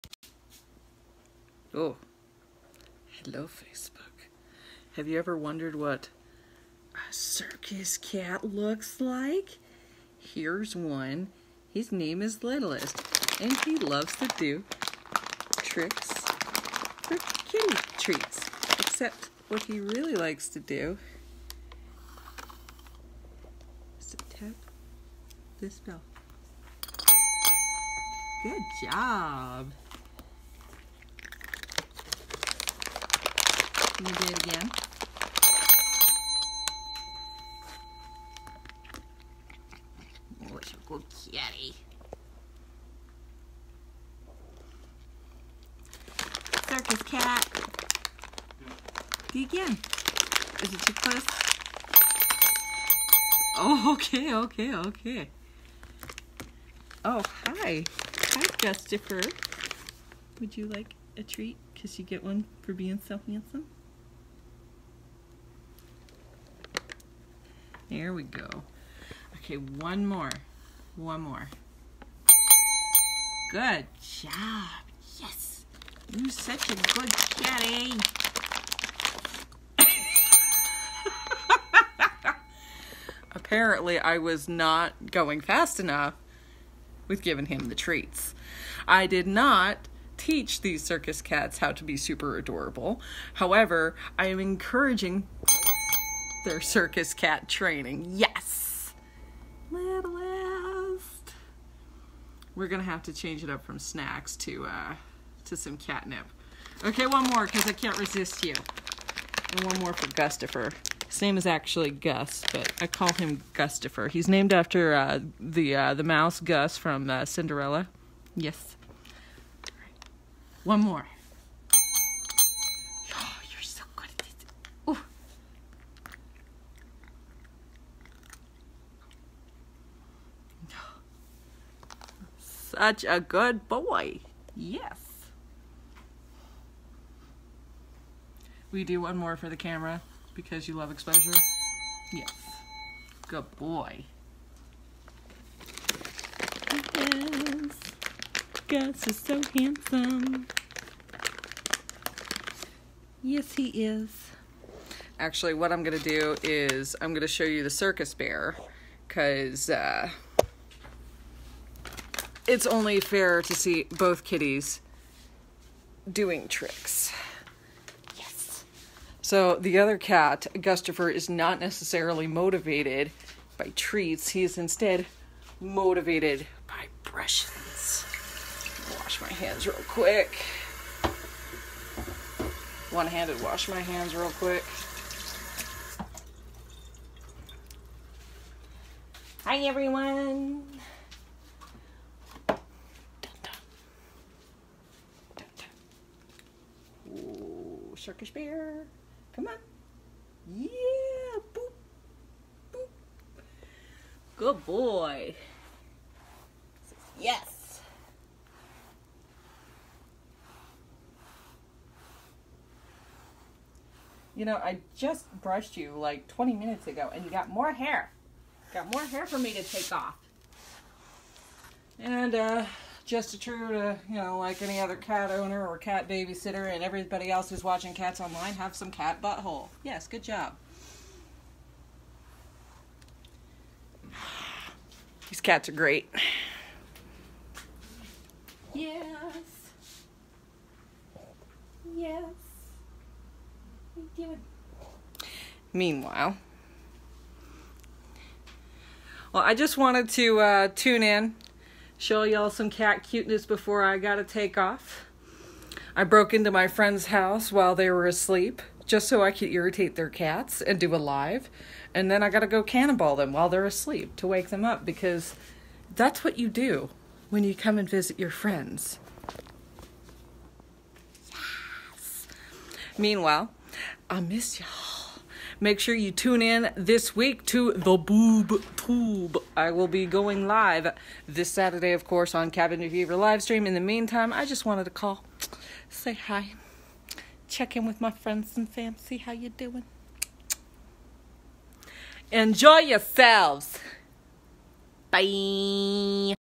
The Amazing Boobzilla encounters Circus Cats
What do you do during the pandemic when your friend needs a ride for work, and you have extra time to kill? You MAKE HIS CATS DO TRICKS FOR THE INTERNET, of course!

The first cat is Littleist! He loves to ring his bell for treats! The second cat is Gus, but I call him Gustopher. He's a sucker for his brush, and will stand on his rear legs while booping my hands to get his brushins!

[video taken during my fb live. You’re welcome.]

Amazing Boobzilla's Boob Toob TWITCH (every Sat @ 7pm CST)-